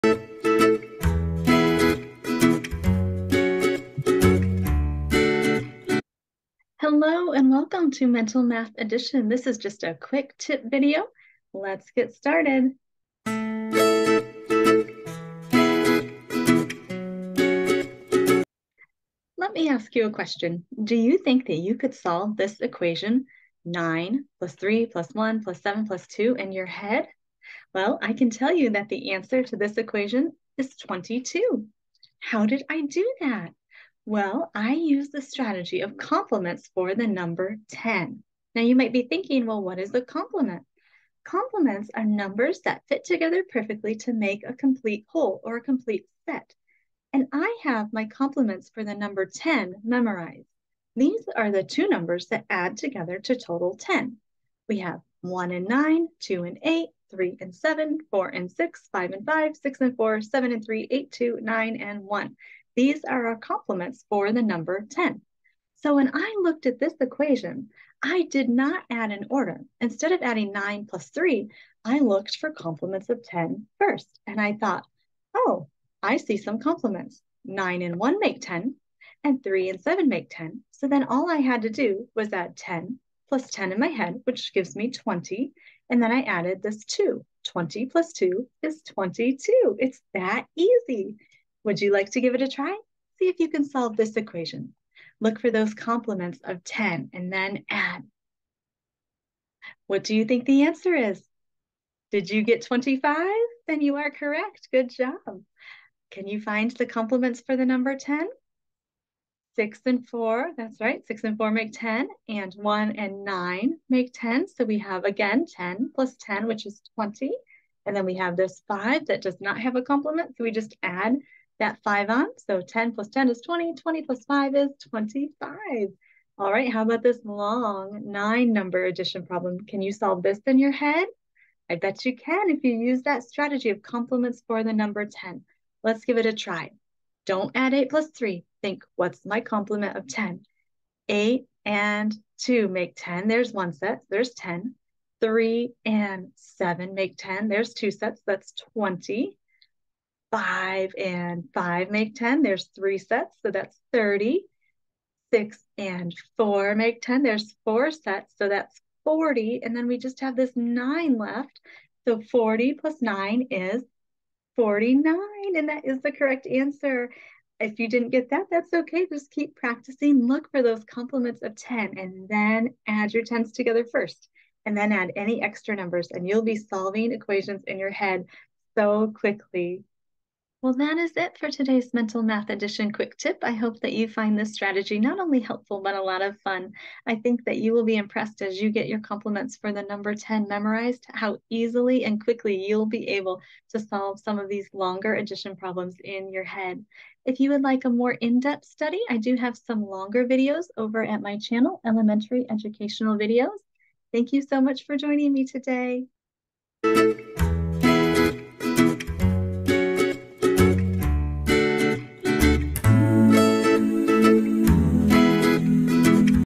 Mental Math Addition - Quick Tip Video
Learn to quickly solve addition problems in your head using the strategy of complements for the number 10. This is a very quick example of how to use this strategy. I also have longer videos that provide more detailed instruction.